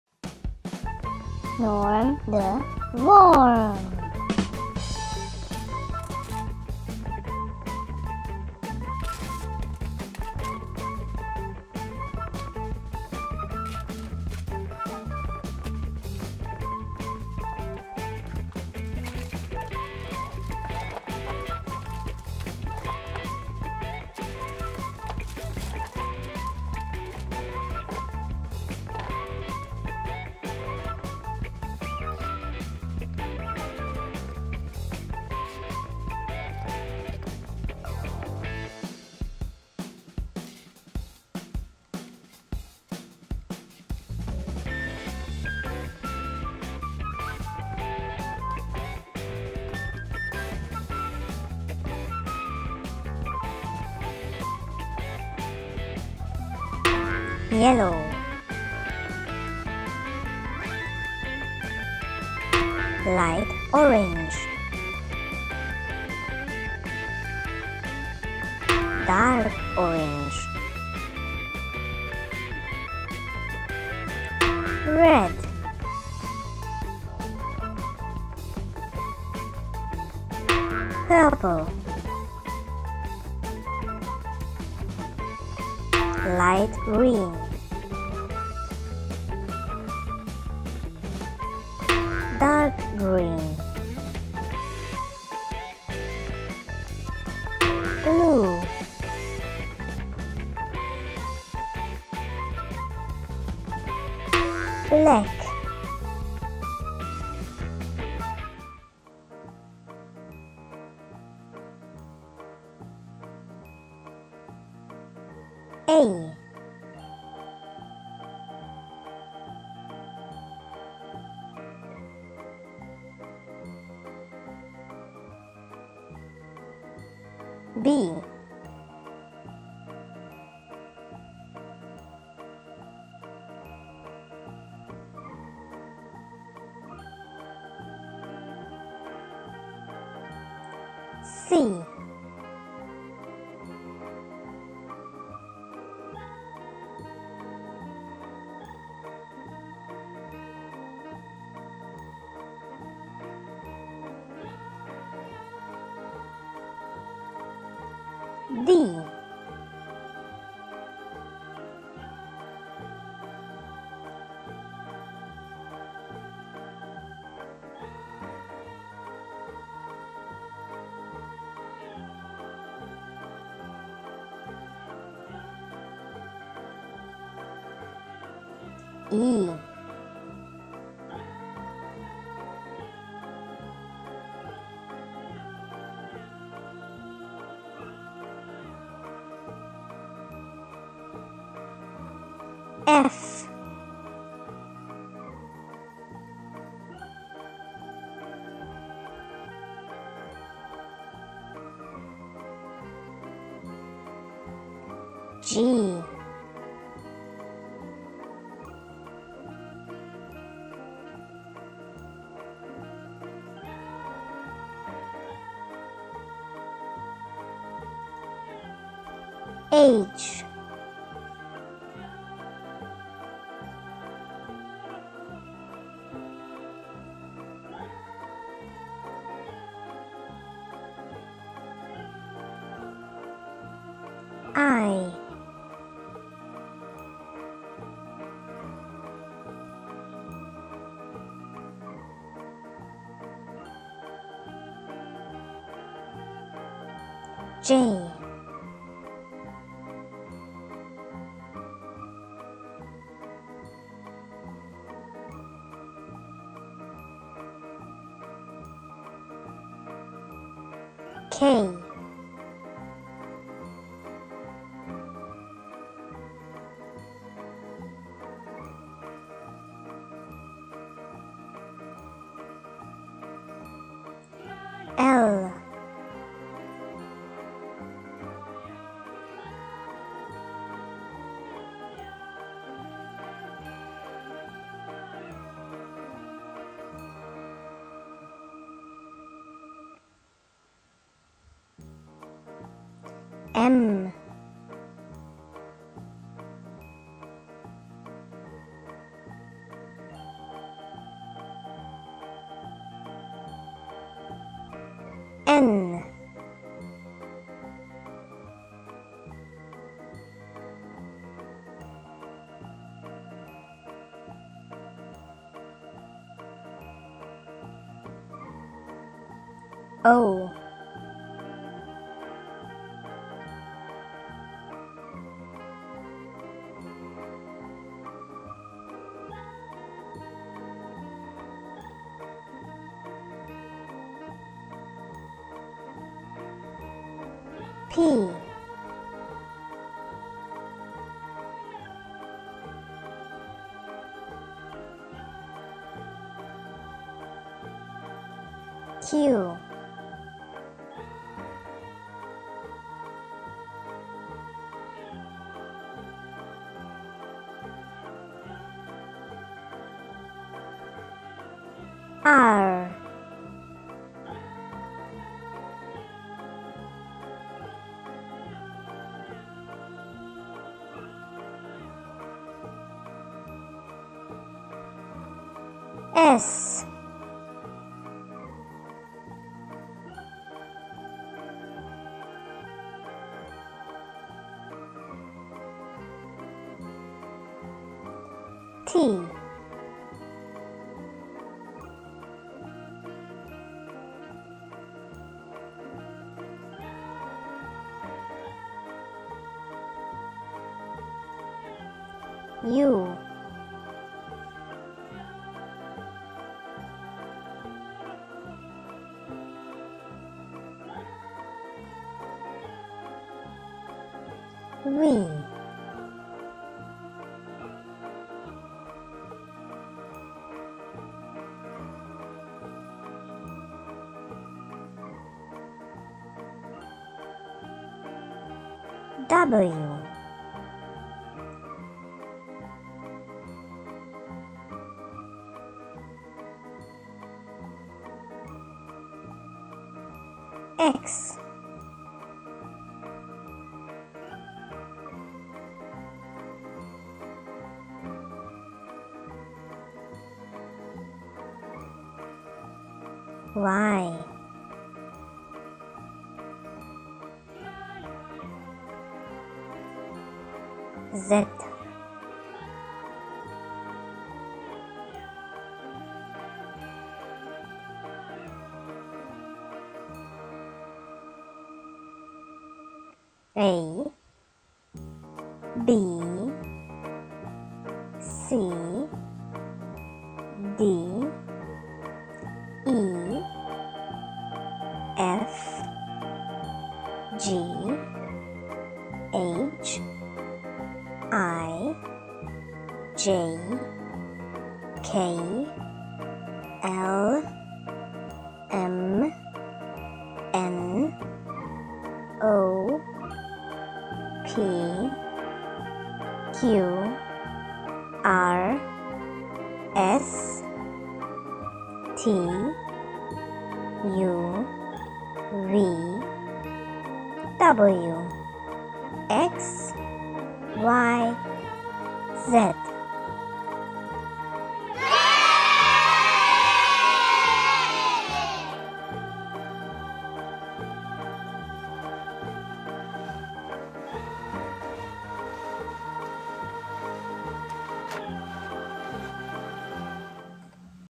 Learn the ABC with a Wooden SNAKE Puzzle | Learn Colors | Fun way to Learn the Alphabet for Kids
A great video to learn the ABC with a SNAKE puzzle! Norm The Worm is helping you.

The content on Norm The Worm IS NOT a production from digitalpuppets.